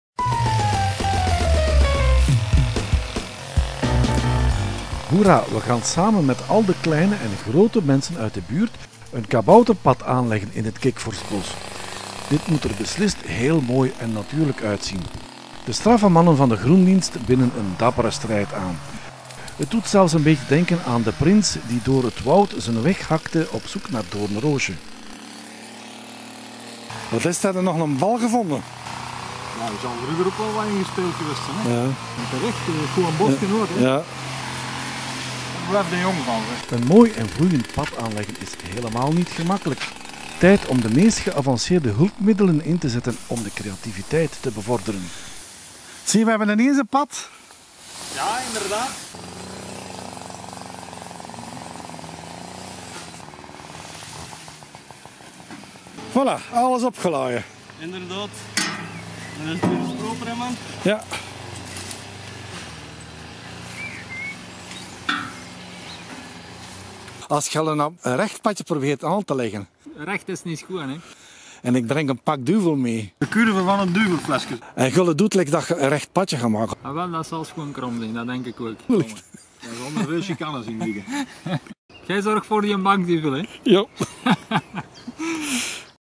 20170508 kikvorstbos groendienst2b
Hoera!
We gaan samen met al de kleine én grote mensen uit de buurt
een kabouterpad aanleggen in het Kikvorstbos!
Dit moet er beslist heel mooi en natuurlijk uit zien...

De straffe mannen van de groendienst
binden een dappere strijd aan.

het doet zelfs een beetje denken aan de prins
die door het woud zijn weg hakte op zoek naar Doornroosje!

Een mooi en vloeiend pad aanleggen is helemaal niet makkelijk!
Tijd om de meest geavanceerde hulpmiddelen in te zetten
om de creativiteit te bevorderen...